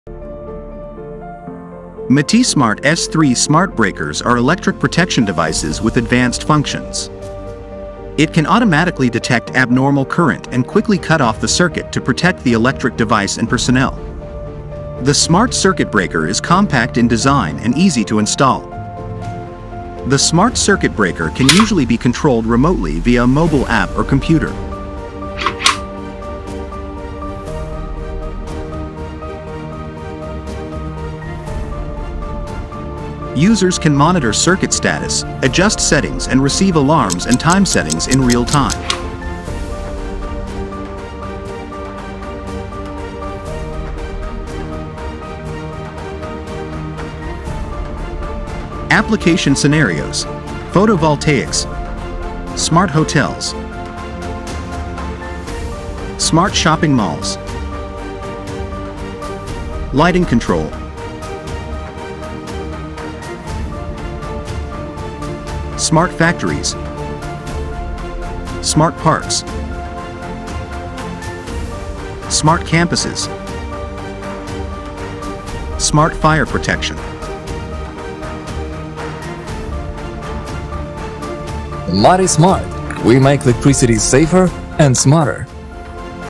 Matismart iMCB / Smart Metering Breaker MTK63(integrated set)
Features:
1. remote control by app and software platform
2. full electrical protection
3. function parameters can be set
4. monitoring and metering in real time
5. power management
6. Provide API interface